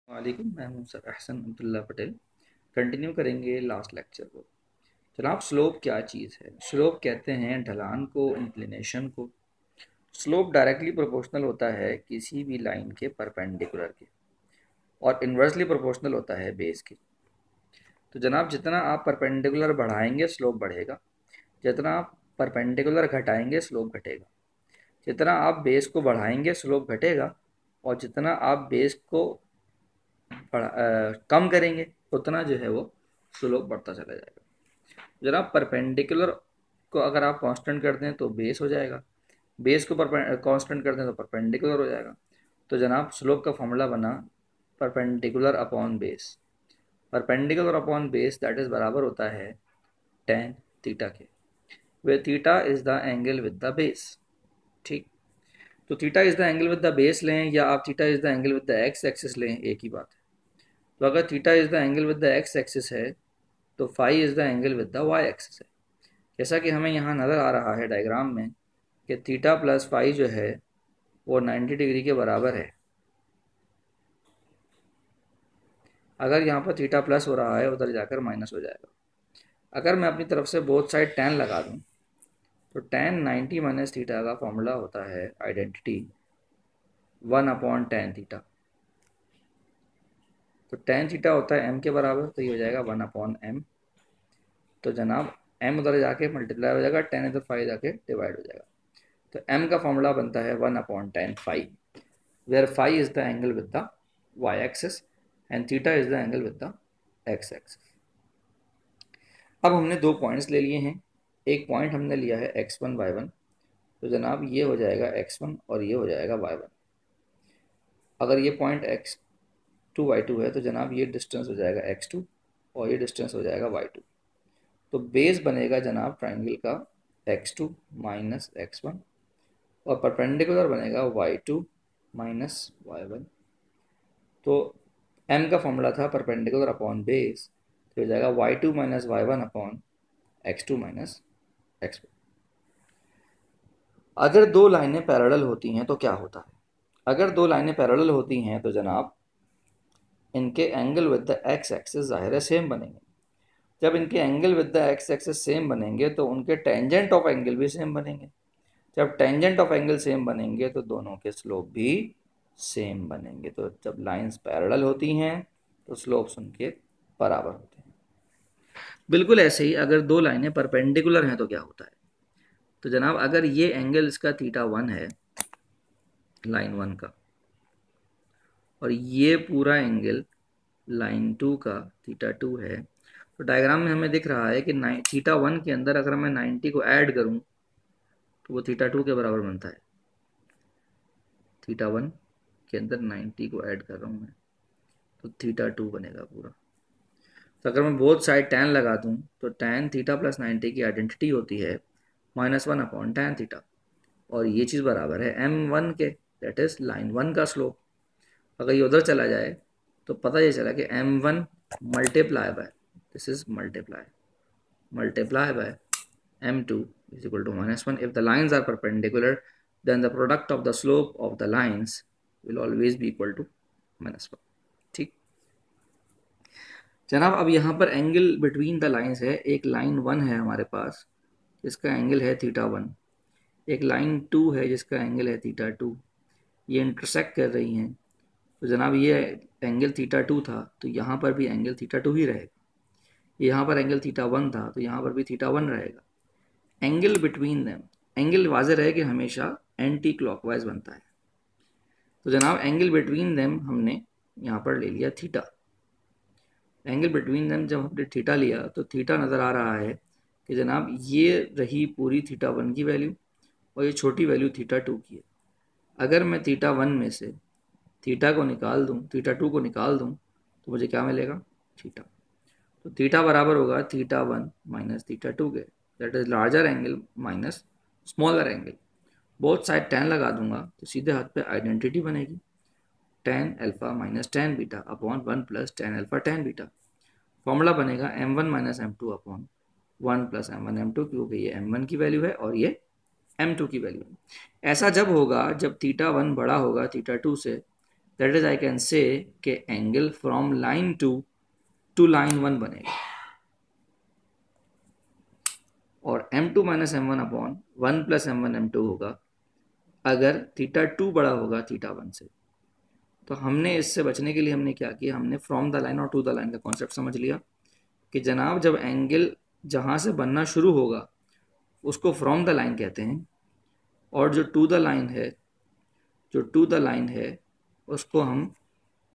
ECAT (APTITUDE TEST) XII Math Chap: Straight Line - Shortcut by Sir Ahsan Abdullah Patel - Part 5
Recommended Quality: 480p or 720p